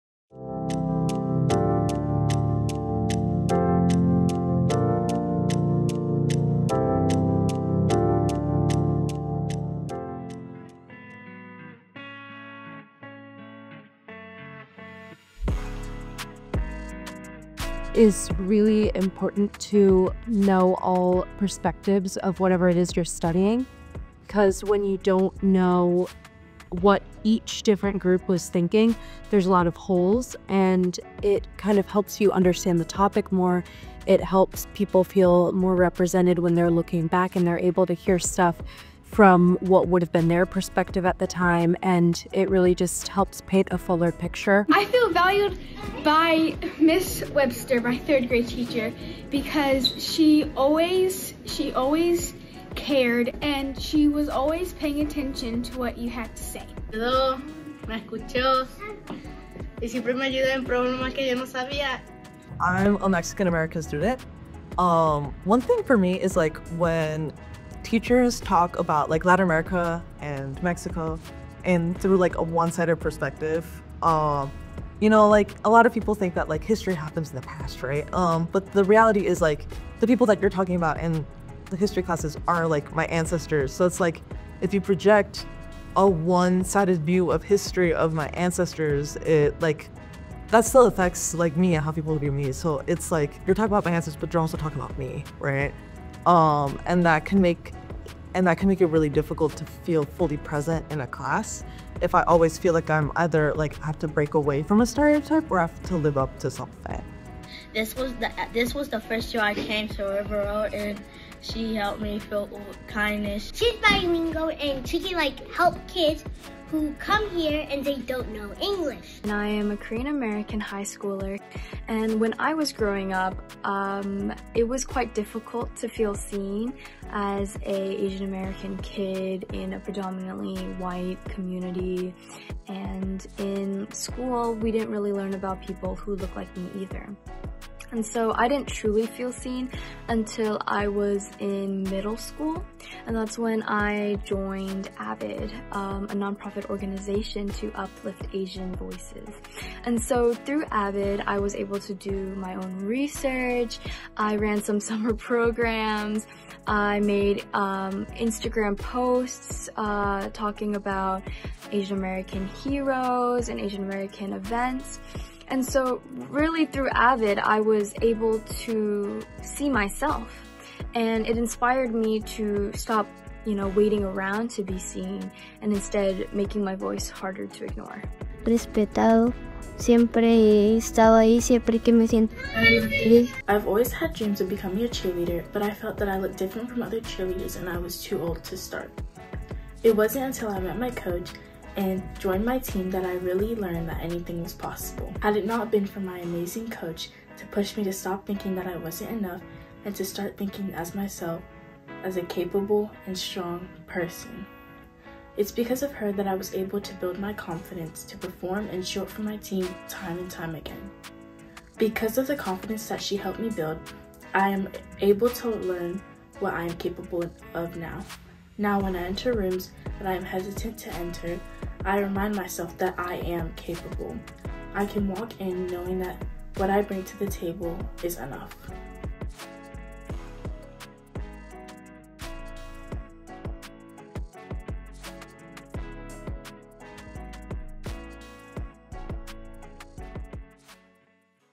When Students Feel Seen and Affirmed in School
We asked students about times they have felt affirmed in school. This is what they shared with us.

Produced by: Chantelle Grace, Jennie Vazquez-Garcia, and Asif Wilson
Edited by: Jill Brinkoetter

i3: Inclusive, Inquiry-Based Social Studies for Illinois
@Social Studies Network